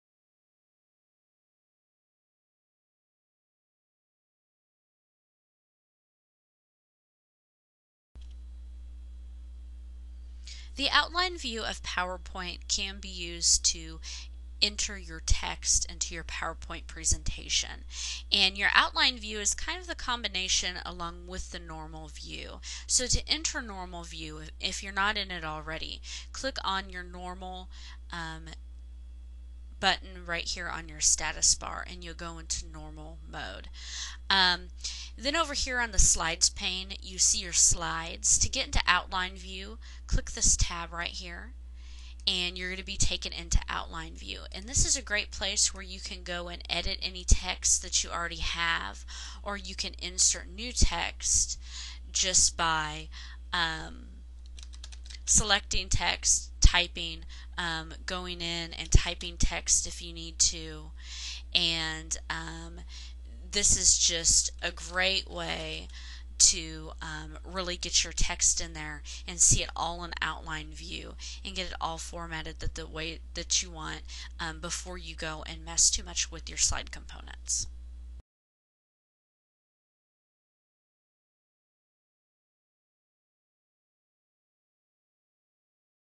Use Outline View in PowerPoint 2007
See a higher quality video on TechTutor.TV! Outline view in Microsoft PowerPoint 2007 can be used to enter the text into your presentation without having the distractions of images or formatting. In this tutorial, you will learn how to use outline view in PowerPoint 2007. See higher quality tutorials all for FREE